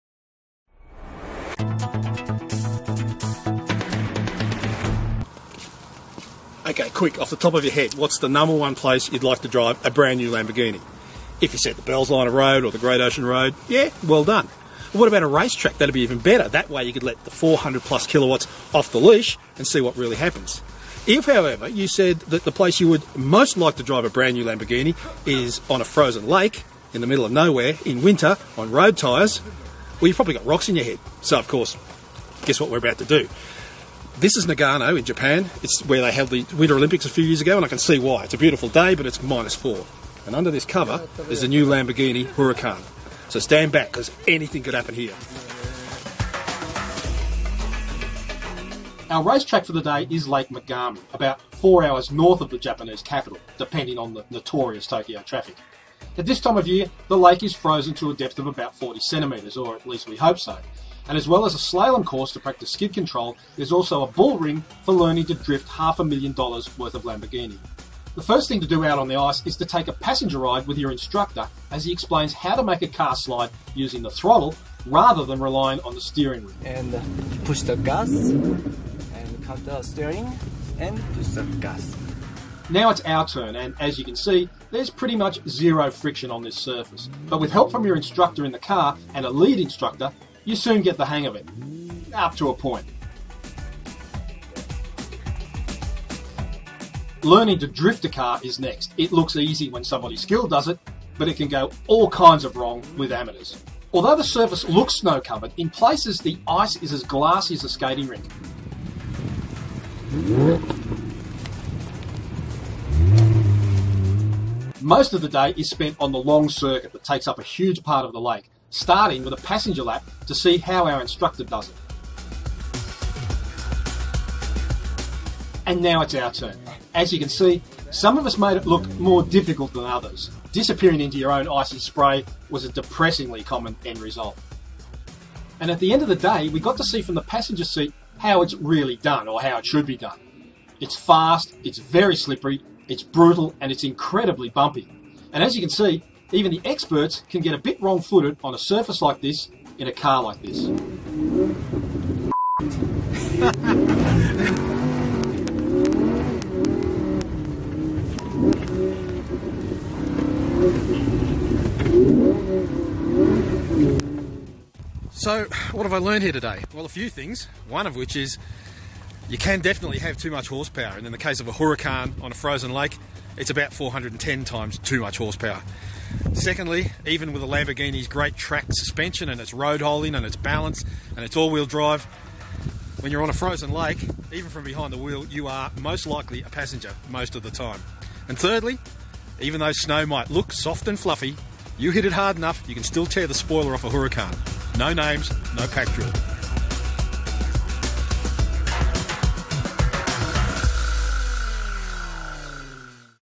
Drifting a Lamborghini Huracan in snow
David Morley discovers drifting a 448kW supercar in the snow is a slippery slop.